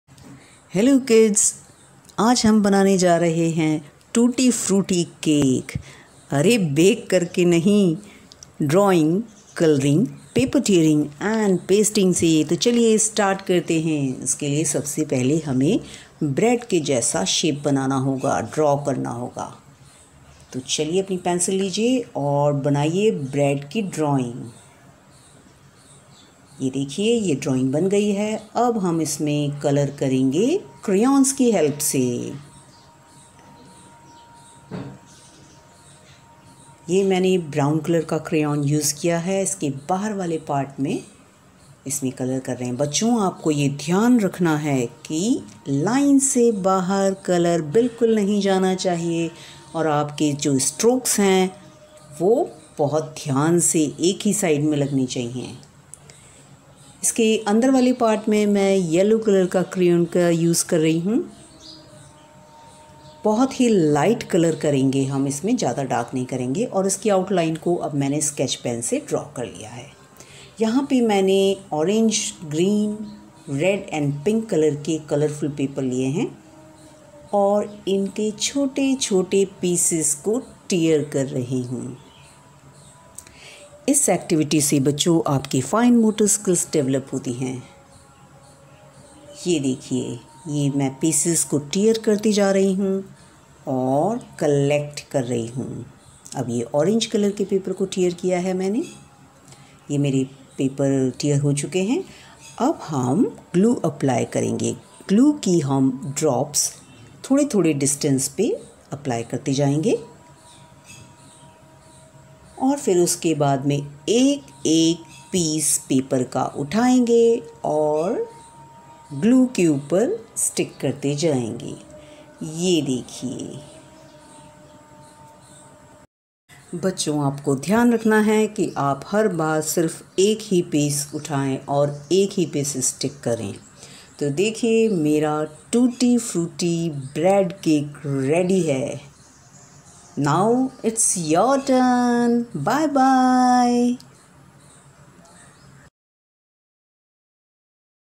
Paper tearing and pasting activity for kids / paper tearing and pasting for kindergarten
Materials used : pencil, crayons, colourful papers, glue and white sheet.
This activity develops fine motor skills ,hand eye coordination, and creativity in kids